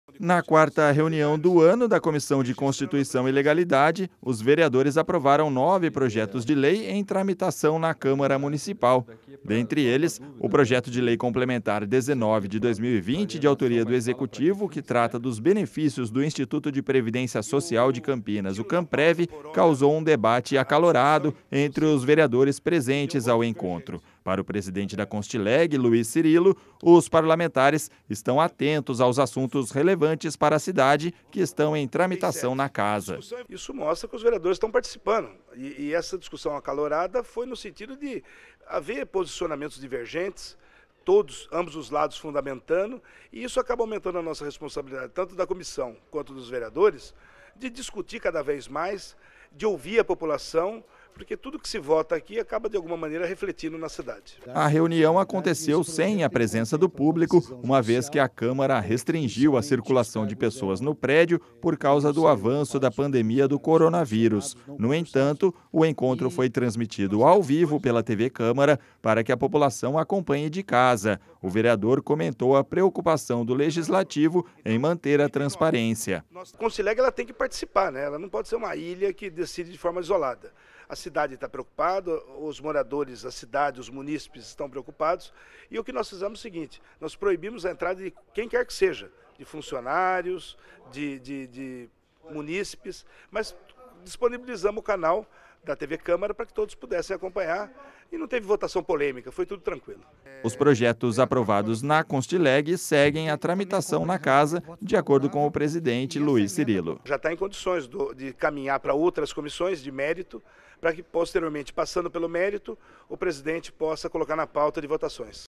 JORNAL CÂMARA NOTÍCIA - Constileg aprova nove projetos em tramitação 19-03-2020
Sem a presença de público por causa das restrições de circulação de pessoas por conta do avanço do Coronavírus, Comissão de Constituição e Legalidade aprova nove projetos em reunião